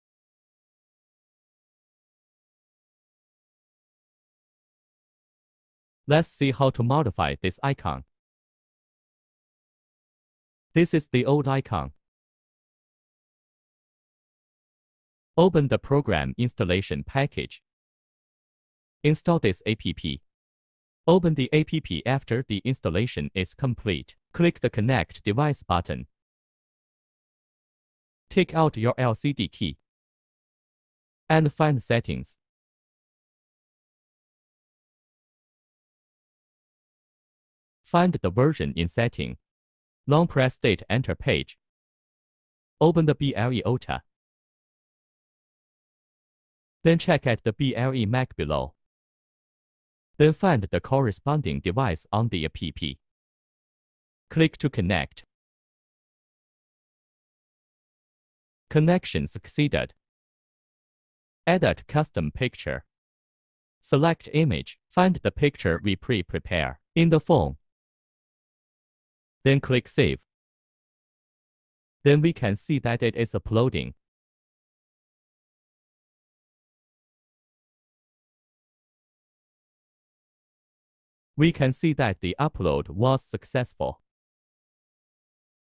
LCD KEY DIY LOGO Uploading for CF920 CF618 CF588 CF828 CF858
This video need installed the APP to binding the LCD key and upload the picture you want, so ask customer services to take the APK.